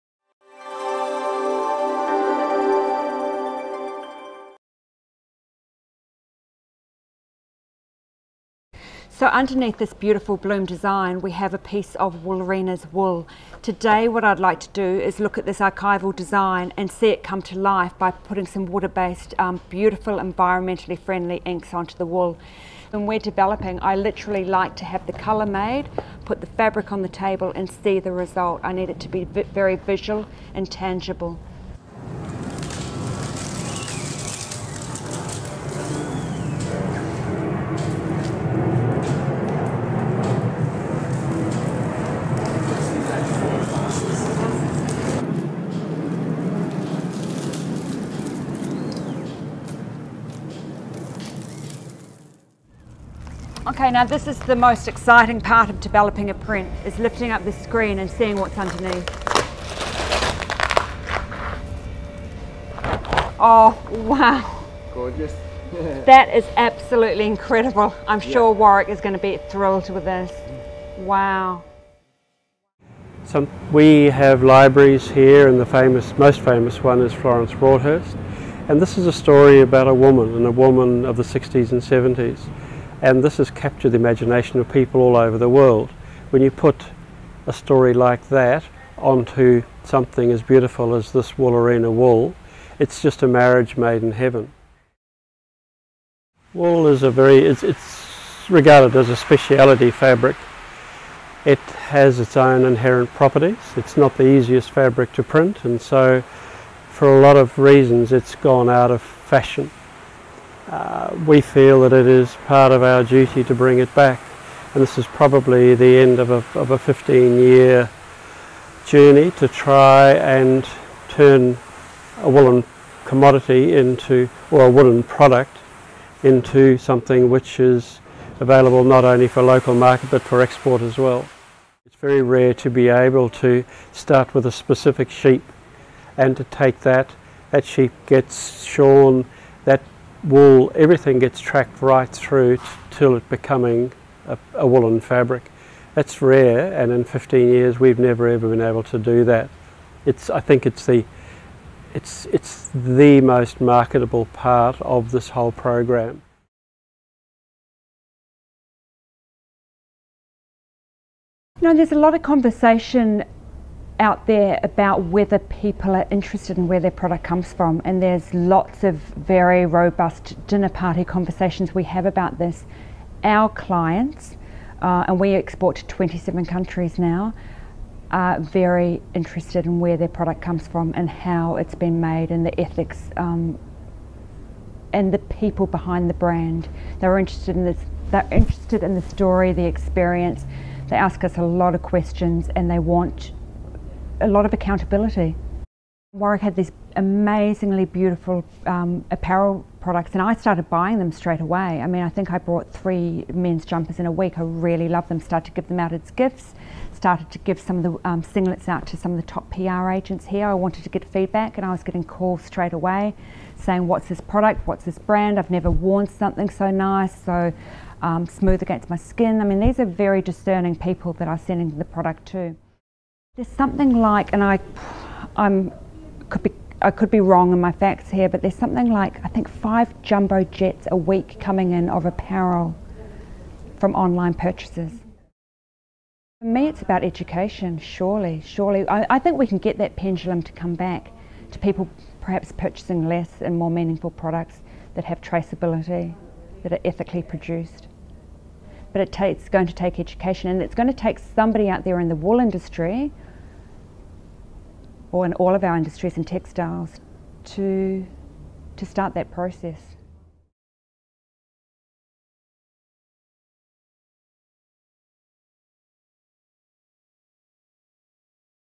Woolerina Signature Prints
Interview by Matt Cawood of The Land Newspaper with David and Helen Lennie.